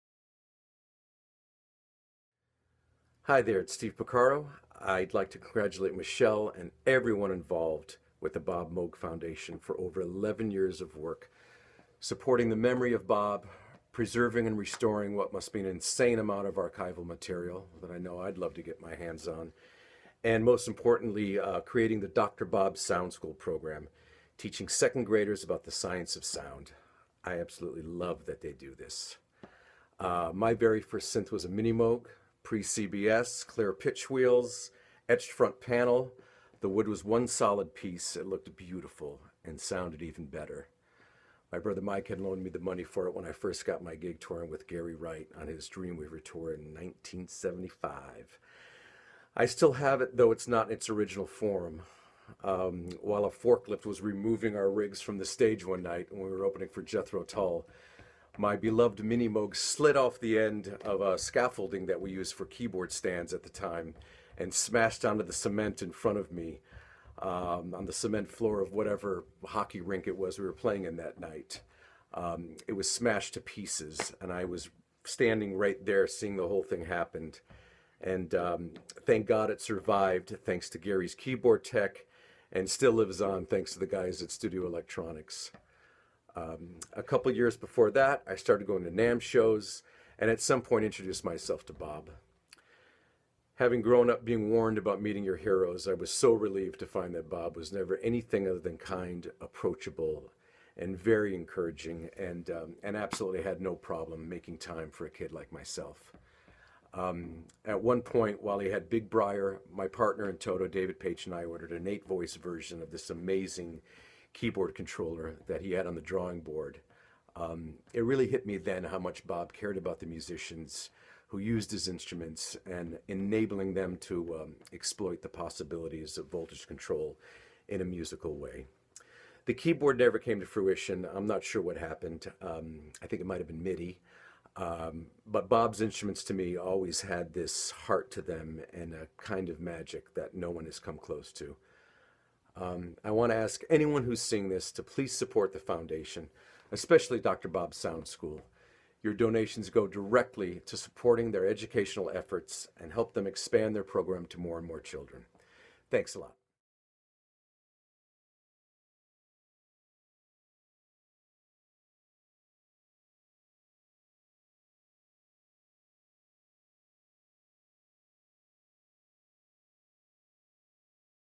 Steve Porcaro on Bob Moog, his Minimoog, and the Bob Moog Foundation
Legendary Keyboardist Steve Porcaro (TOTO) talks about Bob Moog, the important work of the Bob Moog Foundation, and about a mishap his Minimoog had on stage. Please join Steve in supporting the Bob Moog Foundation by donating